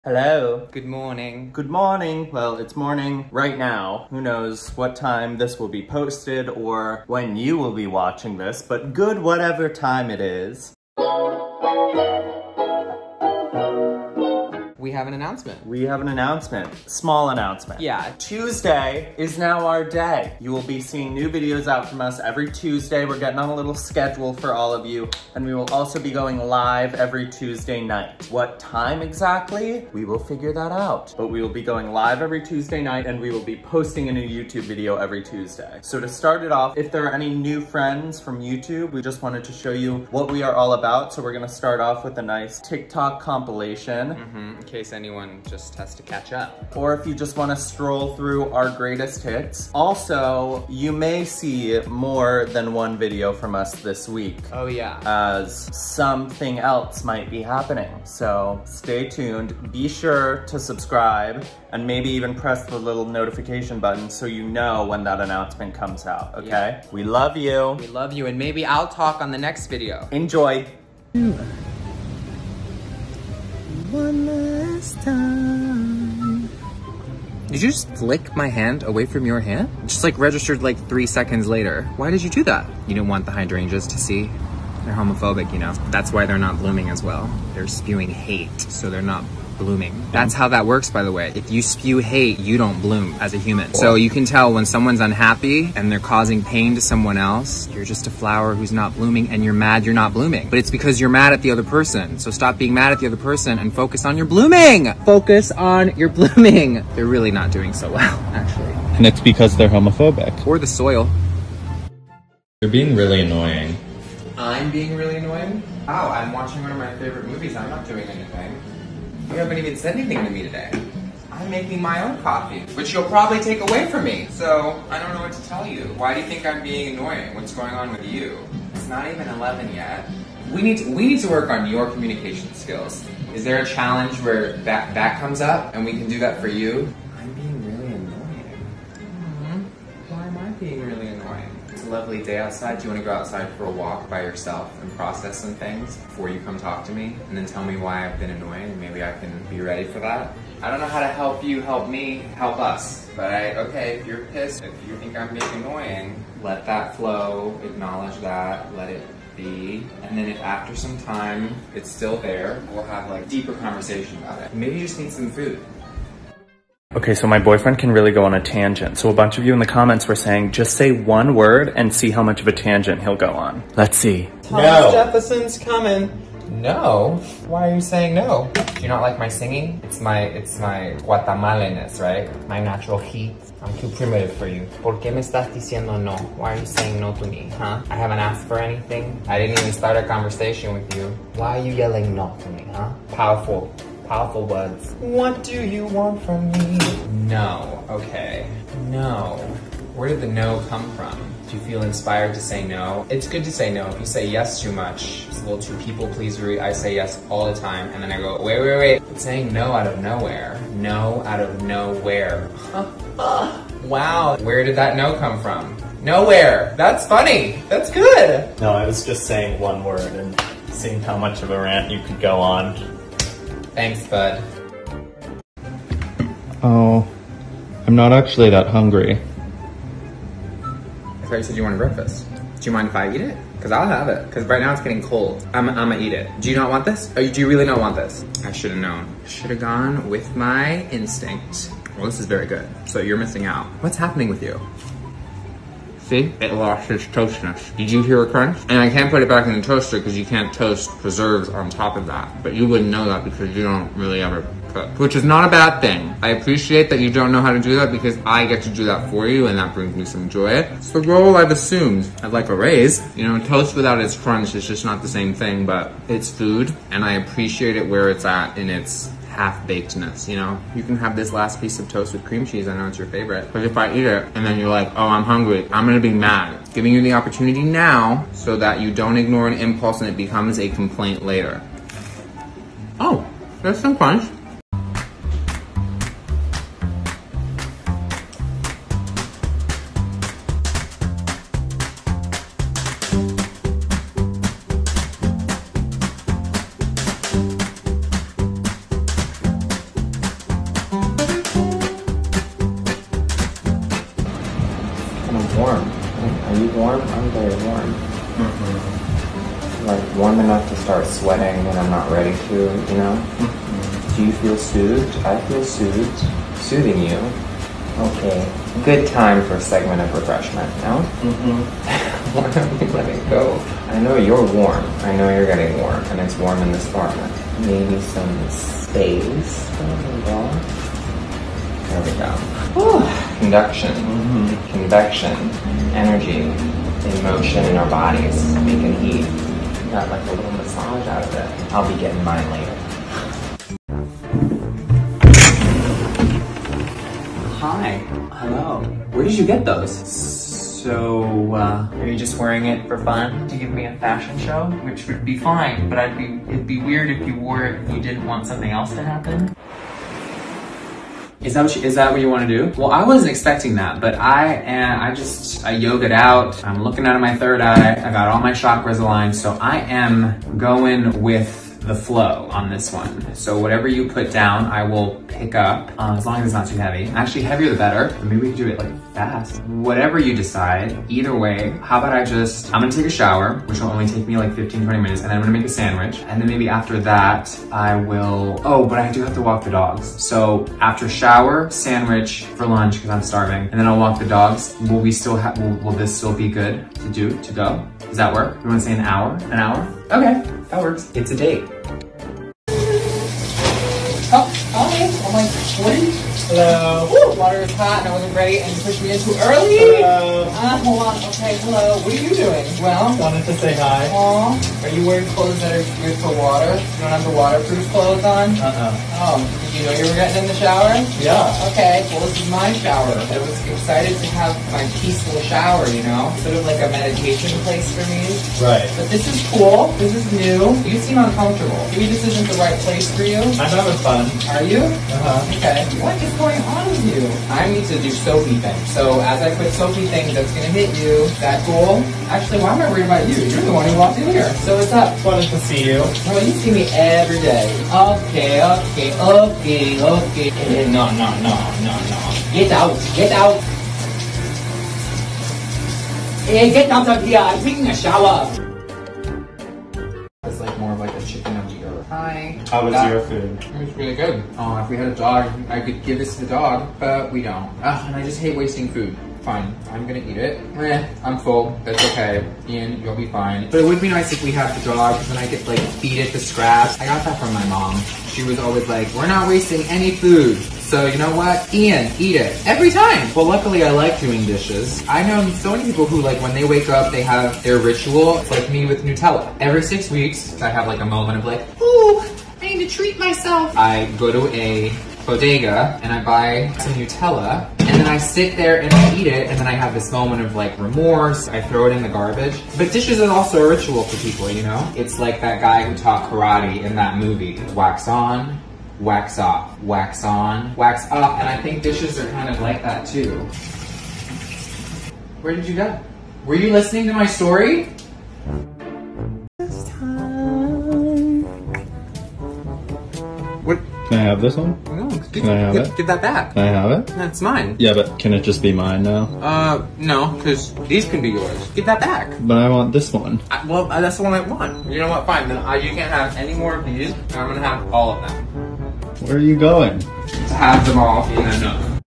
OUR BEST TIKTOKS + ANNOUNCEMENT ;) | CHRIS & IAN
Hiiii friends! We wanted to start off our youtube schedule by putting together our greatest TikTok's and also hint at what exciting things may be coming from Chris and Ian later this week. We listened to your suggestions and coming up soon we have vlogs, get ready with me videos, cooking, and some storytime videos planned that we can't wait to put together for you allll! Also Ian suggested we start calling you guys bloomers due to his "famous" phrase "focus on your blooming". What do you think?? We love you and will see you later this week ;)